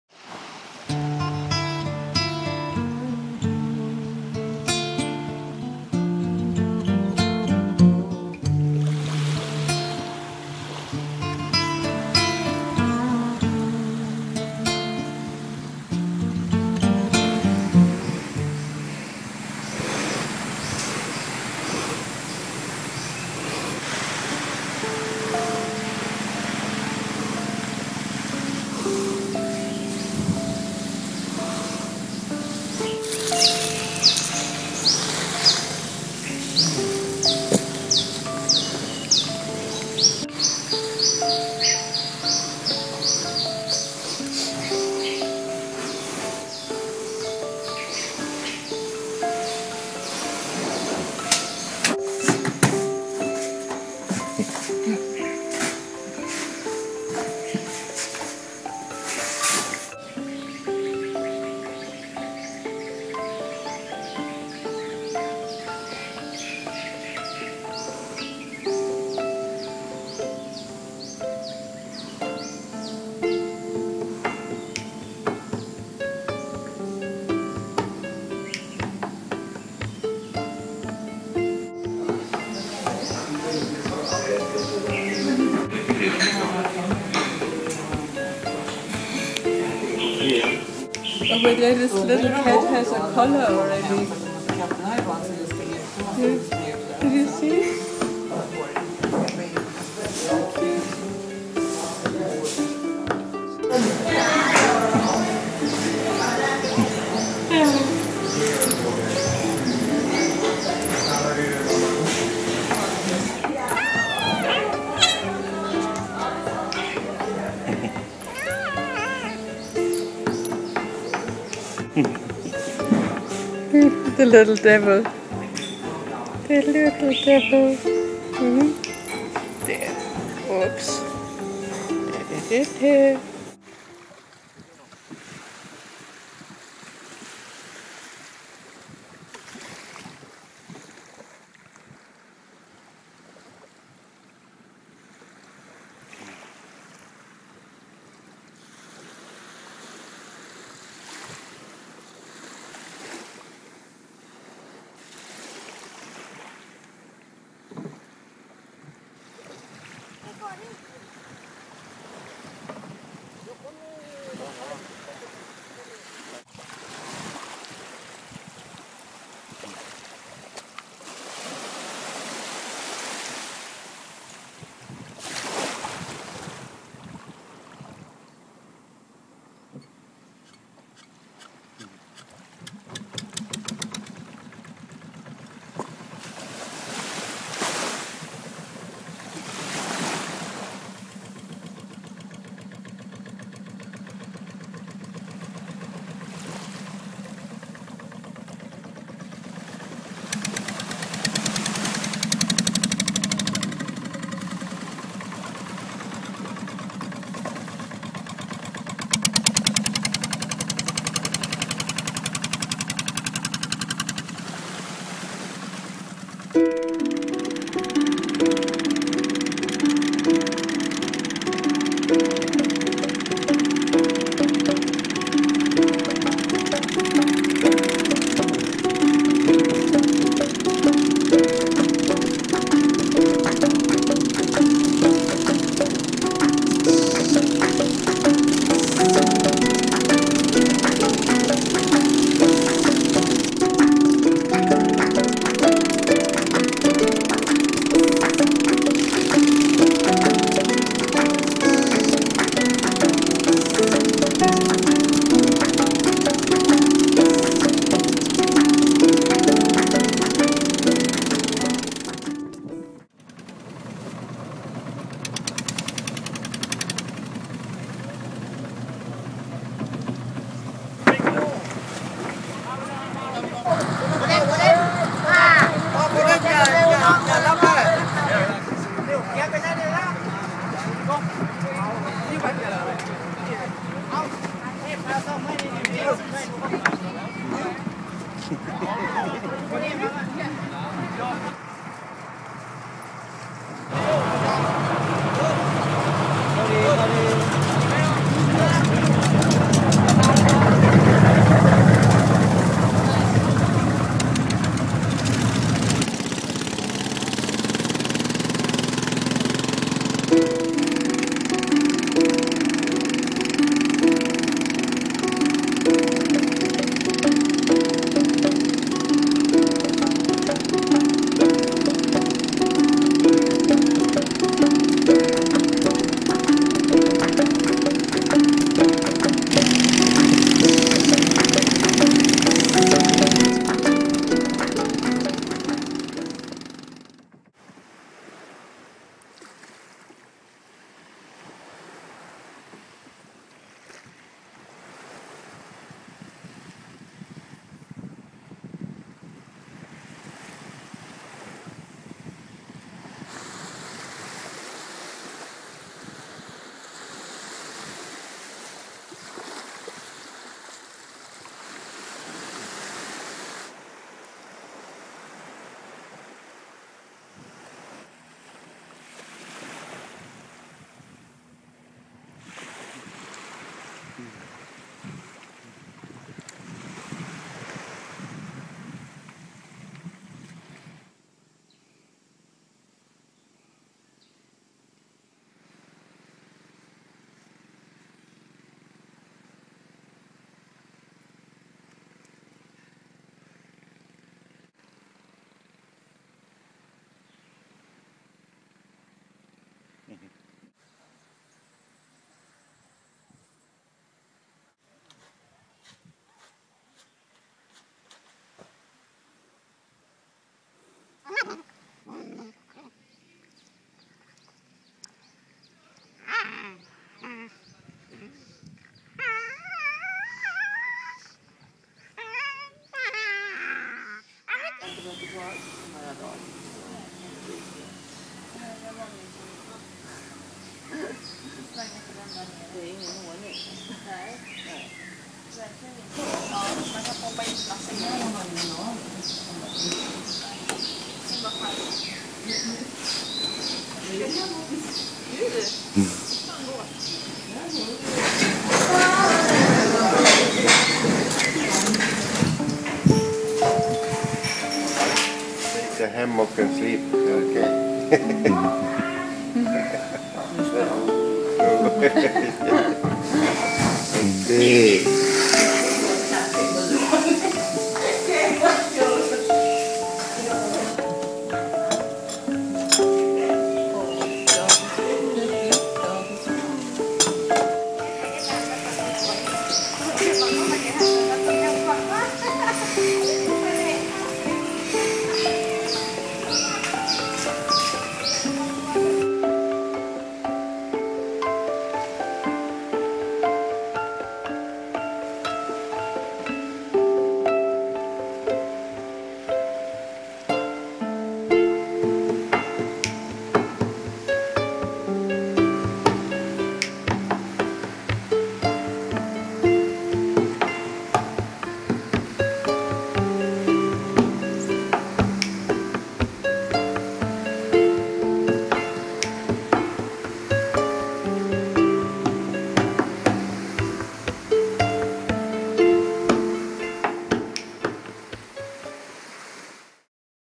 A Day in the Life of New Bungalow
One beautiful day in February 2010 at New Bungalow on the island of Koh Jum in Krabi Province on Thailand's Andaman coast.